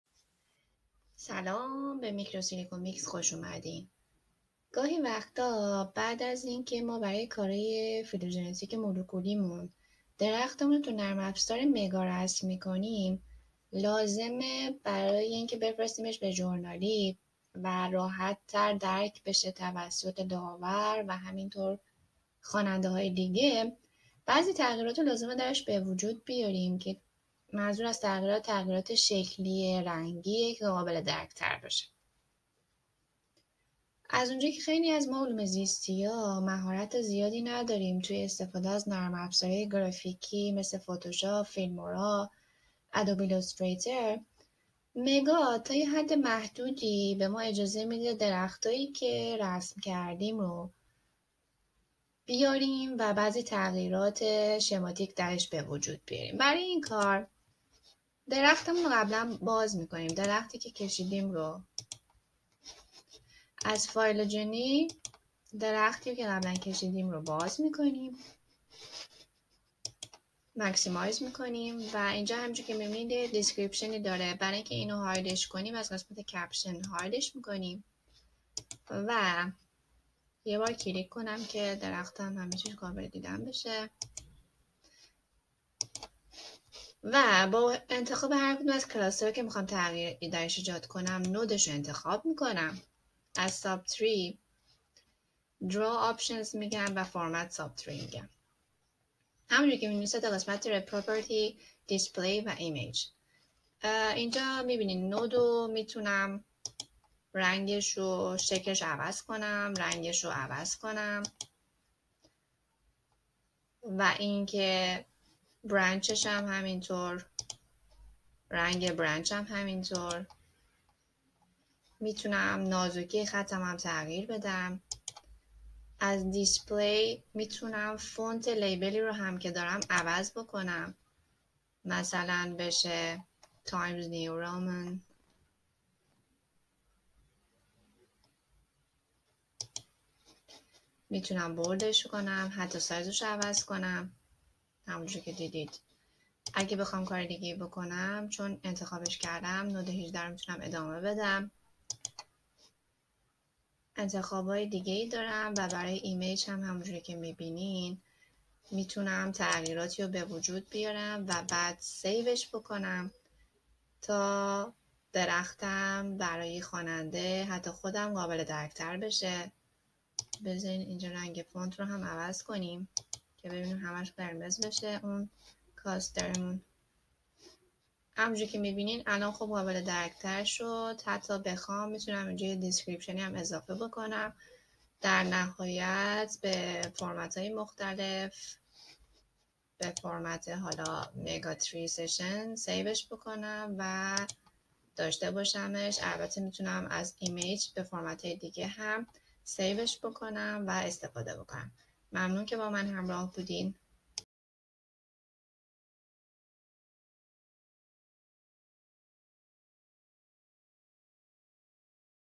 How Can I Edit my Phylogenetic Tree by MEGA Software?
The easiest way to edit you phylogeny!